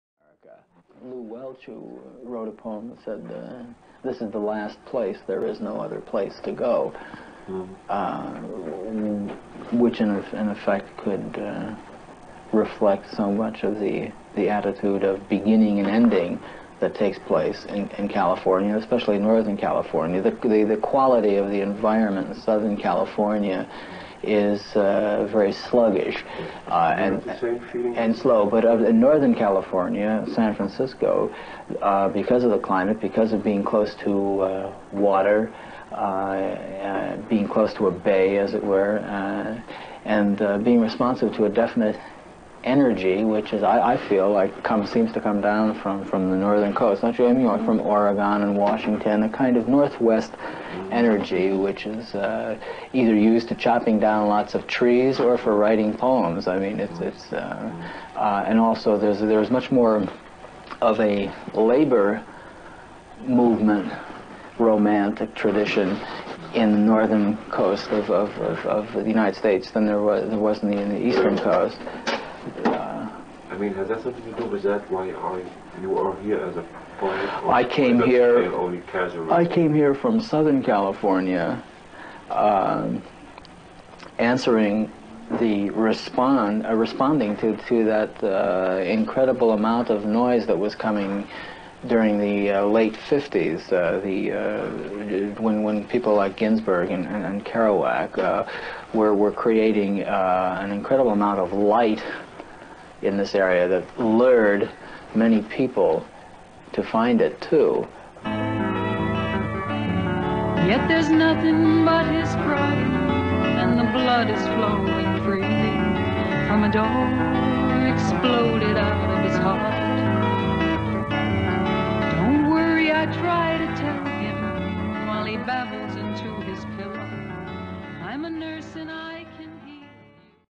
'' david meltzer interview at his home '' - 1969
wonderful interview with the poet singer david Meltzer at his home talking about how san Francisco influences his writing...he was still involved with the group 'serpent power' but problems arose when vic briggs the producer was fired by 'capital records' and that 3rd album was put on the shelf...released a few years back on a swiss label it show's both david & tina on wonderful vocal form.